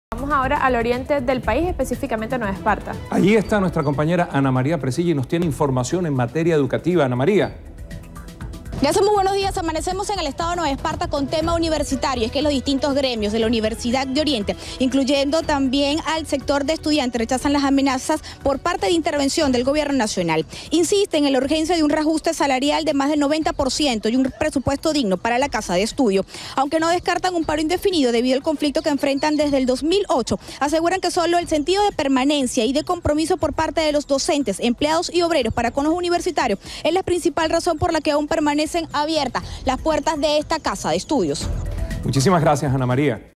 En Nueva Esparta autoridades de la UDO rechazan "amenazas" por parte del Gobierno Nacional
En Nueva Esparta, las autoridades de la Universidad de Oriente, rechazan las amenazas de intervención por parte del Gobierno Nacional.